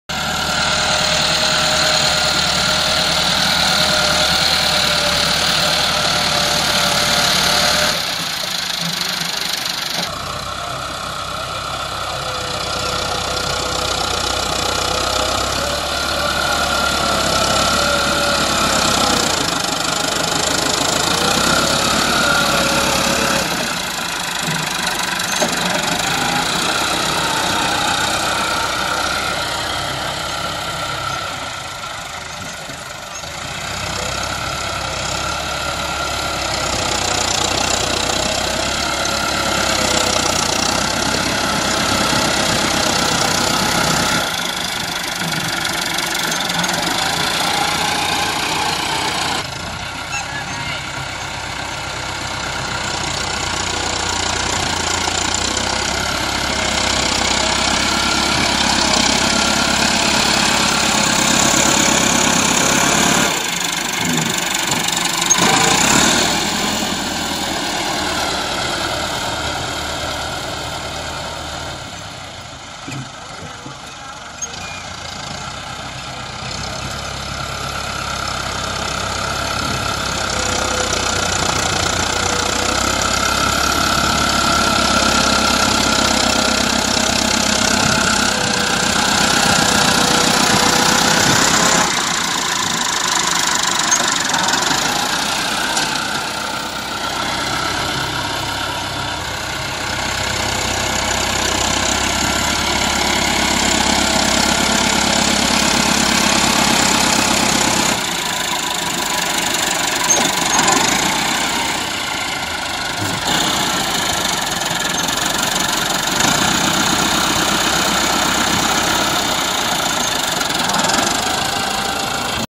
2020 Model Mf 385 performance with front blade | New mf 385 Tractor| Abdul Wahid Khan
2020 Model Mf 385 performance with front blade | New mf 385 Tractor video by Abdul Wahid Khan | Khan Tractor